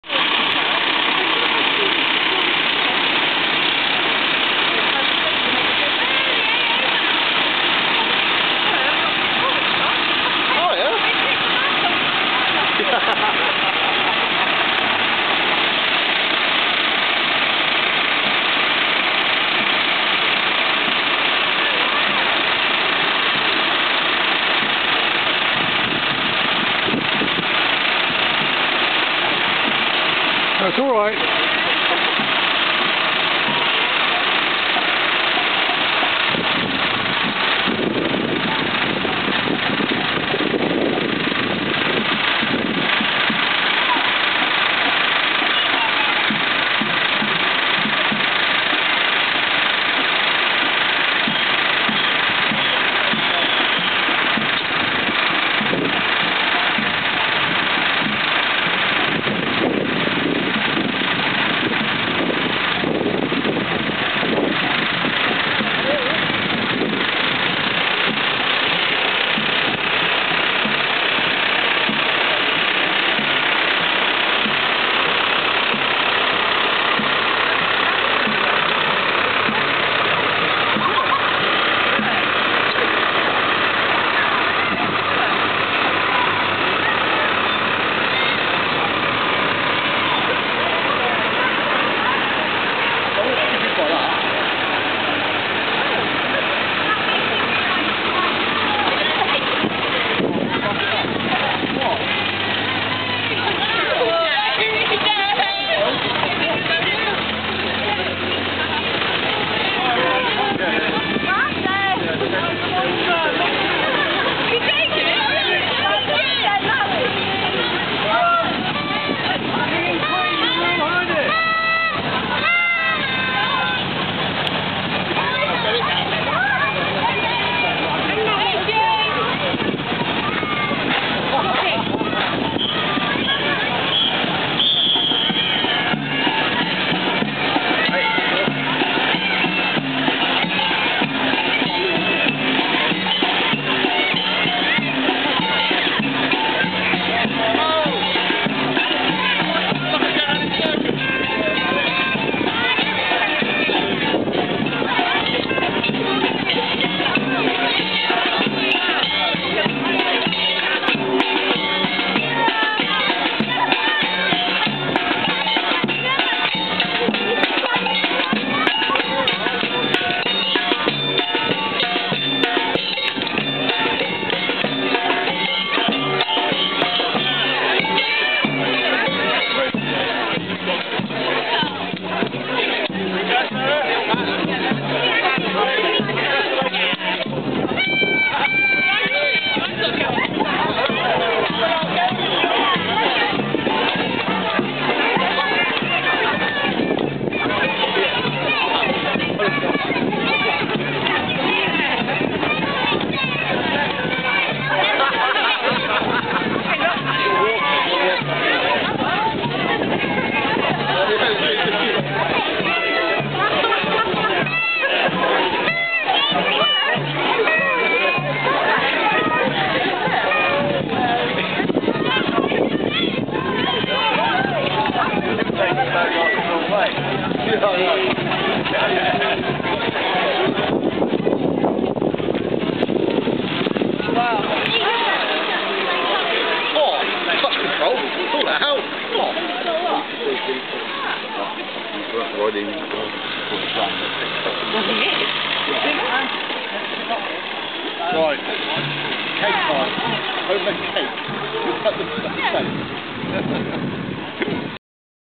Wembury Carnival
Wembury Carnival July 2010 ~ proceeding up Mewstone Avenue